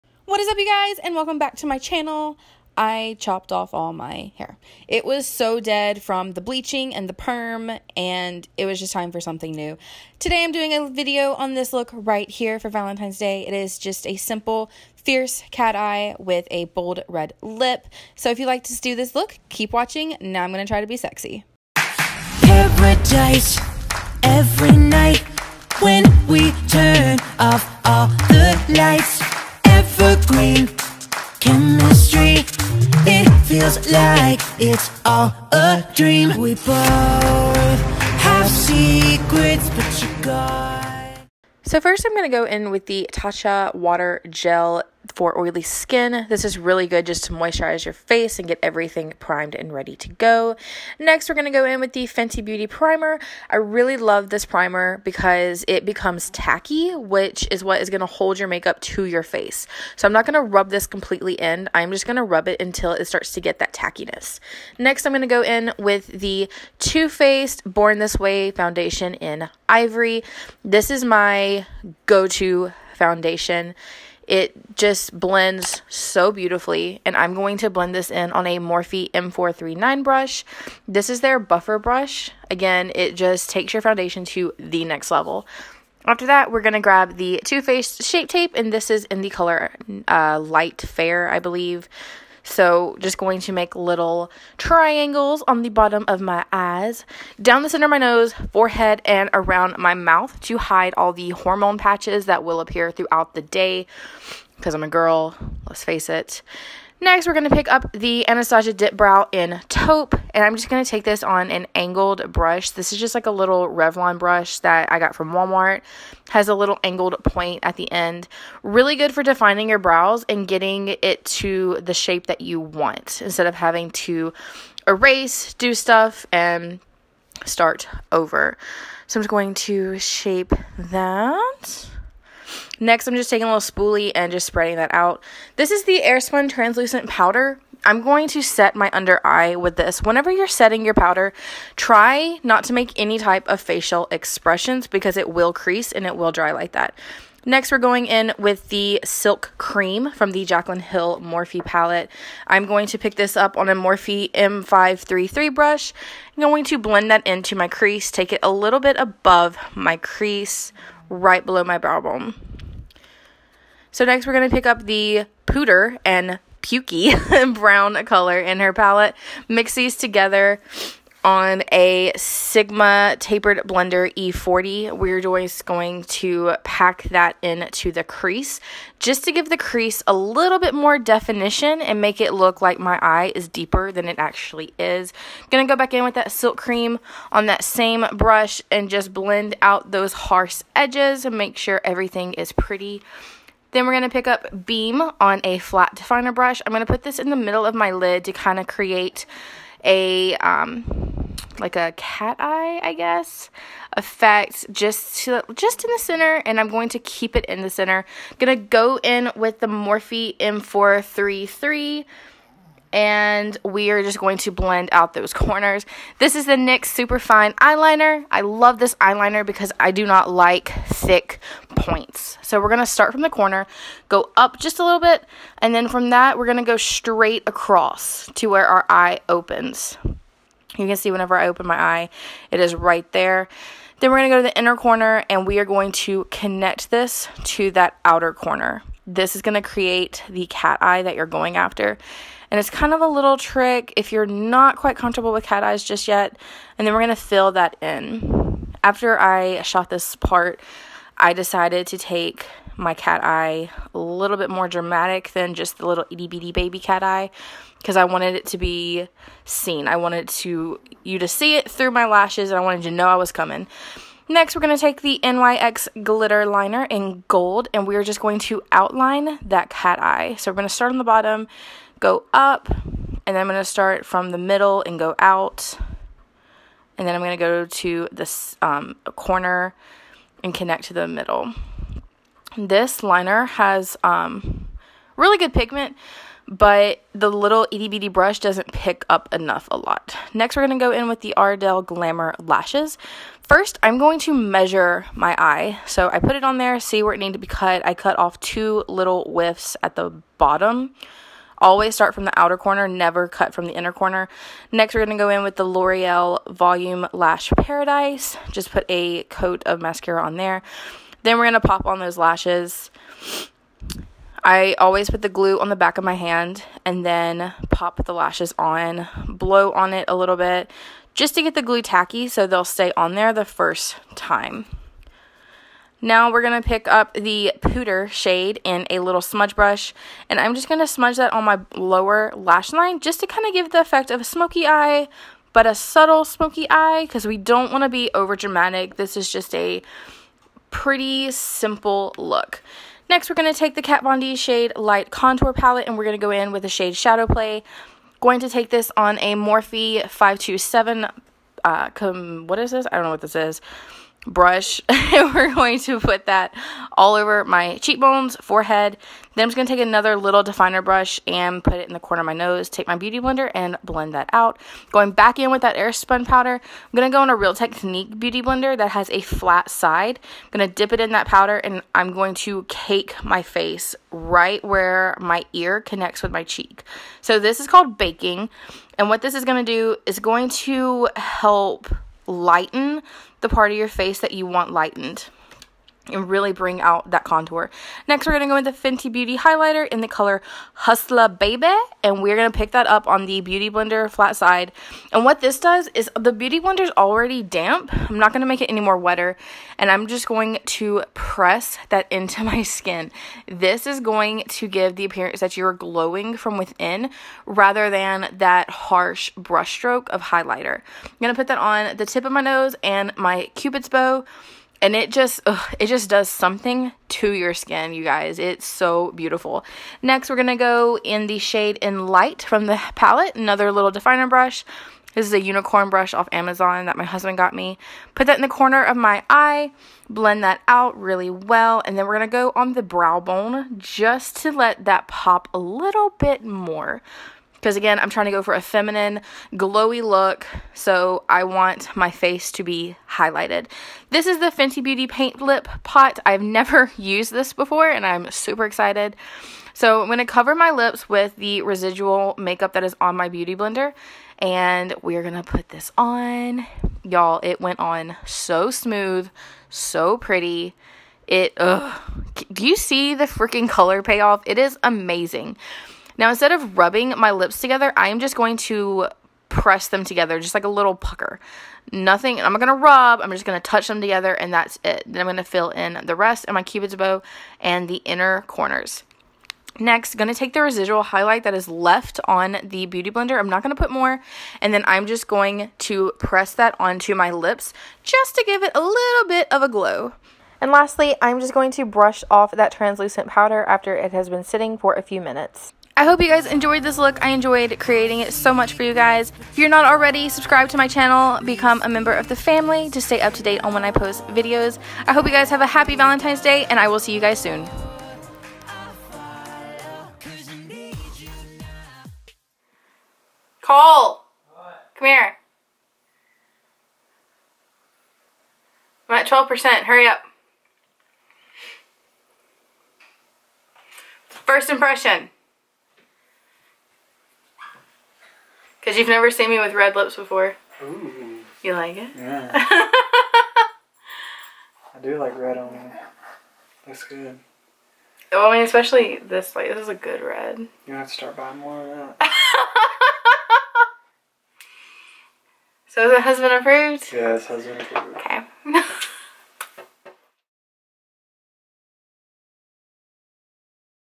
Fierce cat eye with bold lip | Valentines Day Look
What is up you guys! Todays video is all about feeling beautiful and fierce!

Scroll down for product info :)

Products mentioned:

Tatcha Water Gel
Fenty Beauty Primer
Too Faced Born this way-Ivory
Tarte Shapetape-Fair
Airspun Powder- Translucent
NYX Superfine Liner-Black
NYX Glitter Liner-Gold
Loreal Lash Paradise- Black
Ardell Glamour Lashes
Duo Lash Glue
Kat Von D Shade Light Contour pallet
Jaclyn Hill X Morphe Pallet
Fenty Beauty Highlighter- Hustla Baby

Brushes Used:

Morphe
M533
M433
M439
M527
Morphe Beauty Blender
Real Technique Blender
Real Technique powder brush
Sigma E40
Flat definer brush

Stalk me!!